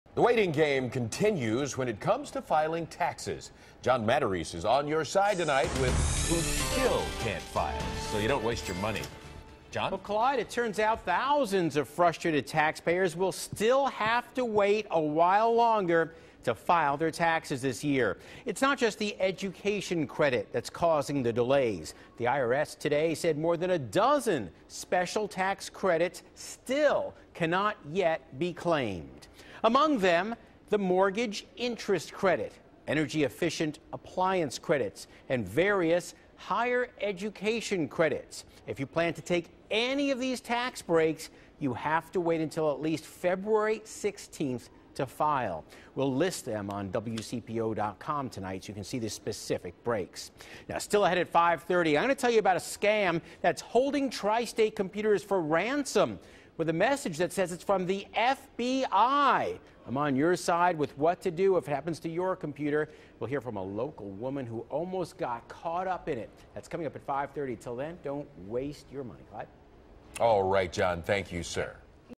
More tax filing delays
John Matarese reports on what may hold you up